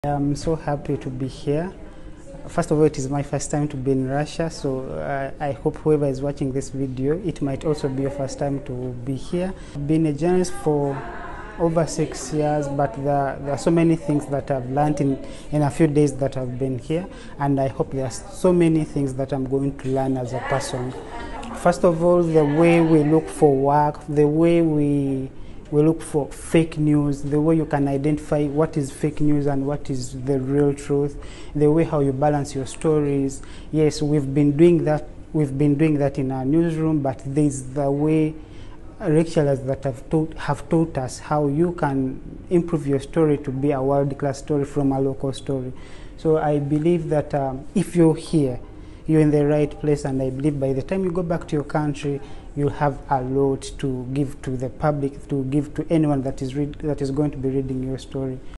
Джон Мусинзе из Уганды. Новое поколение
«Я верю, что к тому времени, когда вы вернетесь в свои страны, вам будет, что дать общественности, что дать всем, кто будет читать ваши истории»

🇺🇬 Это слова Джона Мусинзе из Уганды. Он работает журналистом уже шесть лет. Однако за пару дней стажировки в рамках программы «Новое поколение» он узнал много нового, что поможет оптимизировать его журналистскую деятельность.

📝 По его словам, можно не только научиться фактчекингу, но и понять, как в локальных новостях выделять основную информацию, чтобы донести ее до мировой общественности.